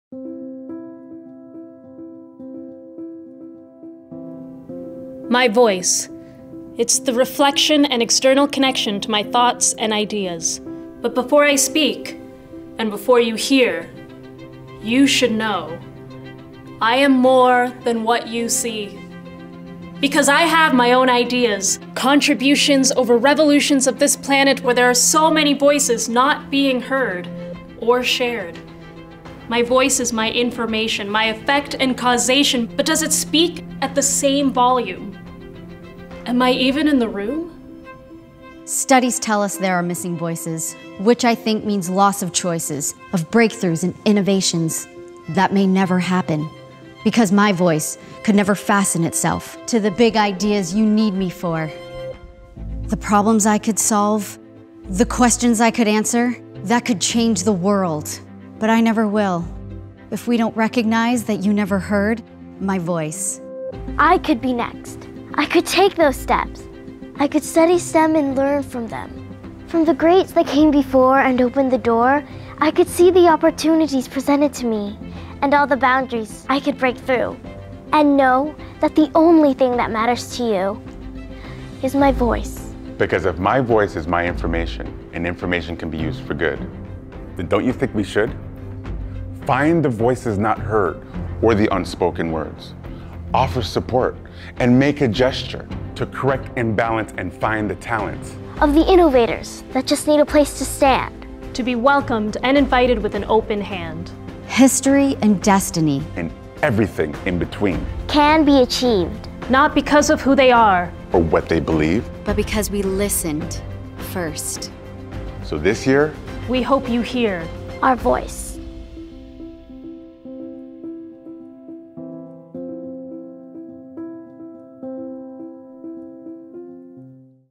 Our Voice
Every individual’s voice deserves to be represented and heard. Only when we promote and support women and diversity, can we become a gender-balanced world. If your voice is your information, how will you use it as a force for good?